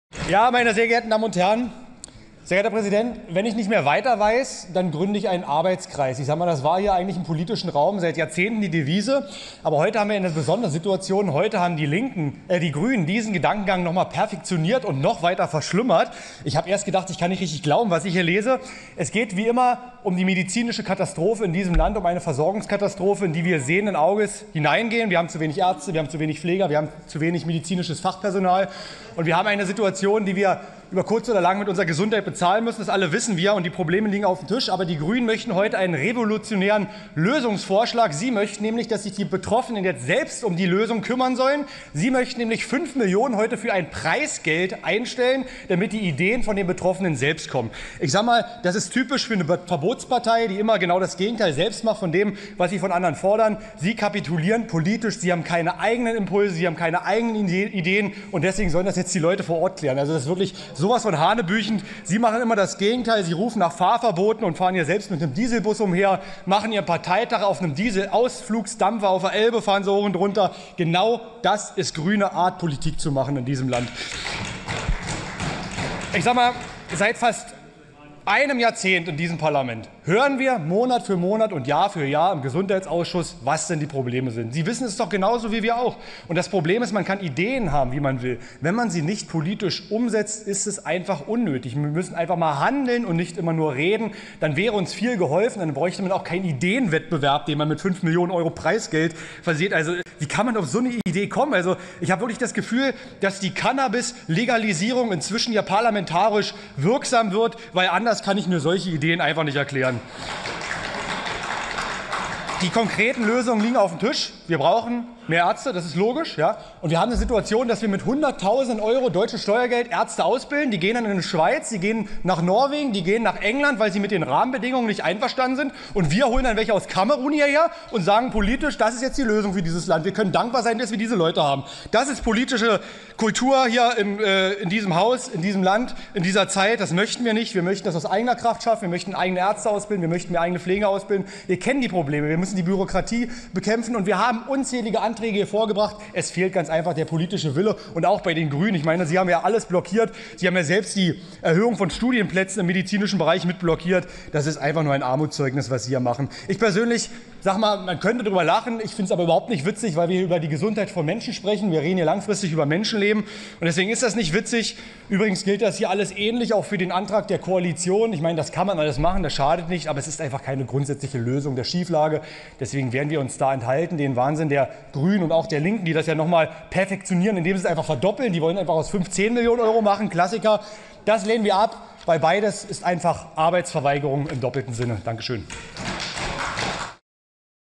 Siegmund: Medizinische Katastrophe abwenden - Ursachen bekämpfen!
Die Grünen möchten einen Ideenwettbewerb für innovative Versorgungsmodelle ausloben, die sowohl auf Prävention als auch auf die direkte medizinische Versorgung abzielen. Dieser absurden Idee erteilt UlrichSiegmund eine klare Absage. „Zu wenig Ärzte, zu wenig Pfleger, zu wenig medizinisches Fachpersonal: Unser Land steuert auf eine medizinische Katastrophe zu. Diese Situation werden wir über kurz oder lang mit unserer Gesundheit bezahlen. Da hilft auch kein grüner revolutionärer Lösungsvorschlag, der mit fünf Millionen Euro Preisgeld dotiert ist.“